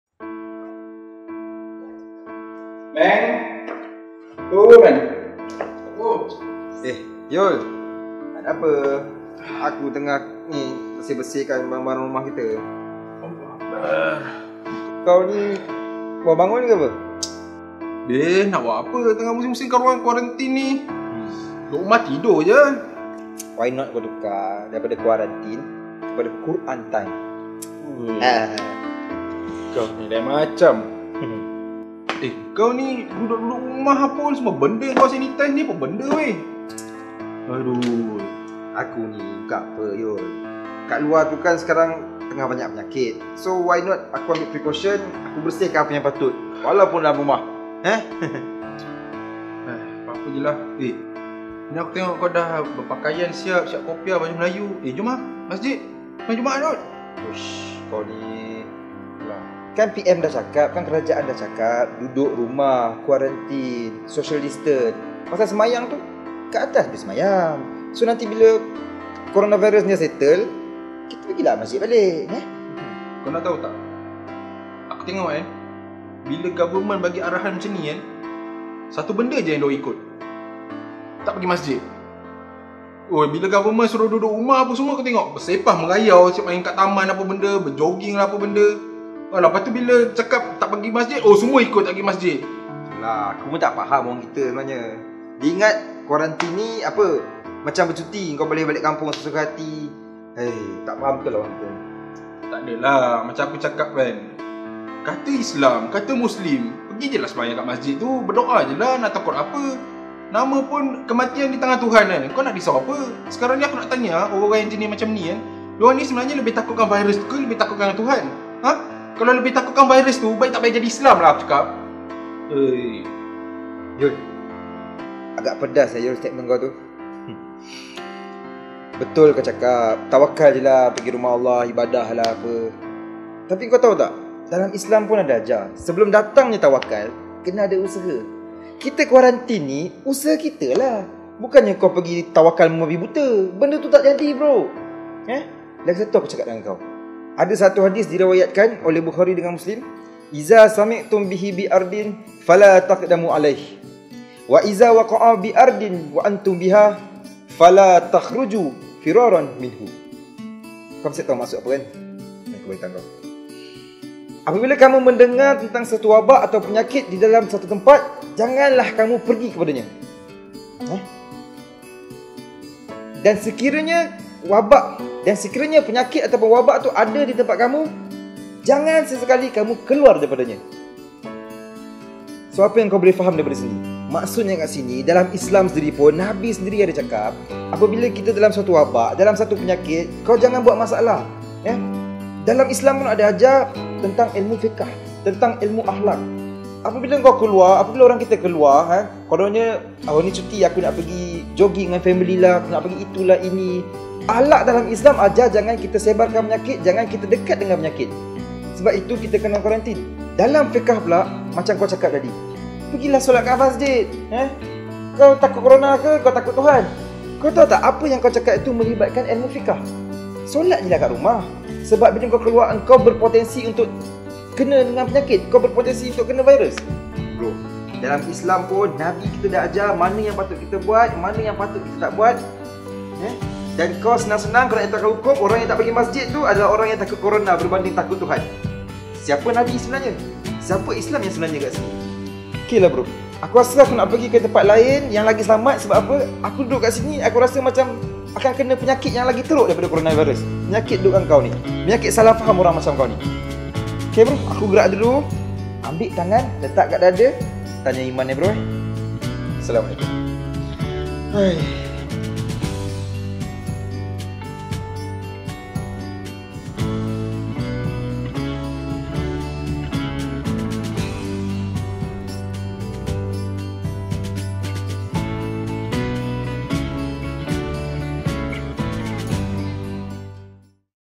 Takut Siapa Sebenarnya Ni ?
Kami di sini mohon kepada kawan-kawan di luar sana supaya sentiasa dengar arahan yang telah dikeluarkan ya, n paling penting jangan sesekali buat spekulasi sesuka hati

Follow FB kami sebab shortfilm akan publish di sini sahaja lepas ni